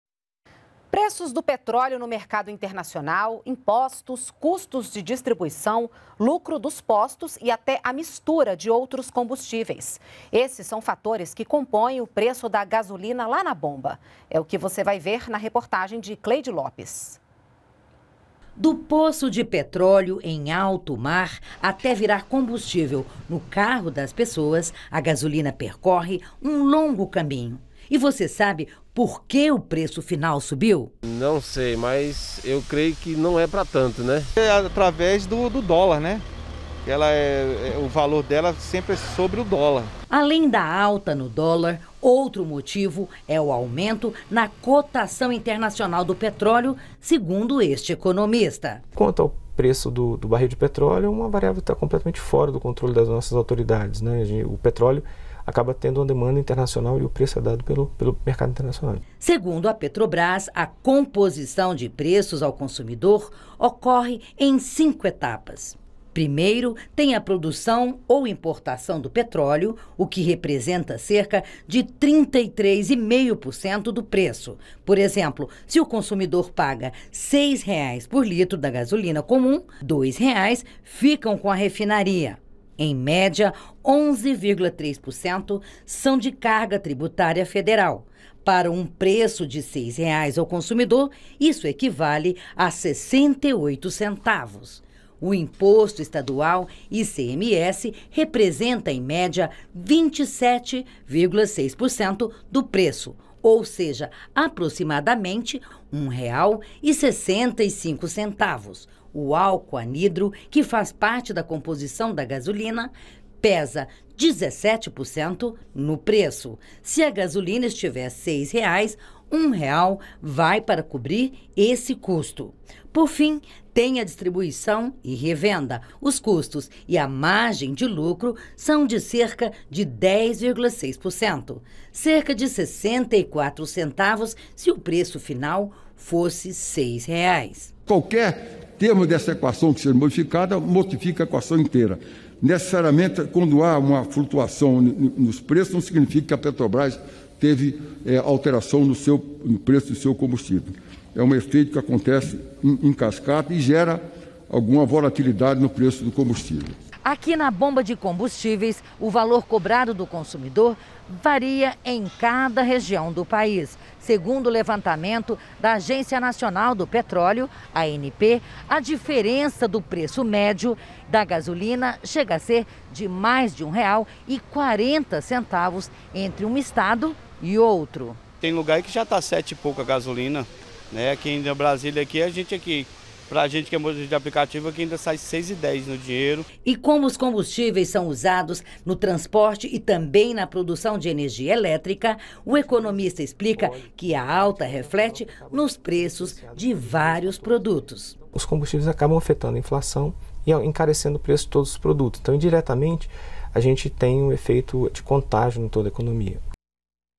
Preço da gasolina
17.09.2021 –  Preços do petróleo no mercado internacional, impostos, custos de distribuição, lucro dos postos e até a mistura de outros combustíveis. Esses são fatores que compõem o preço da gasolina lá na bomba. Entenda na reportagem de Cleide Lopes.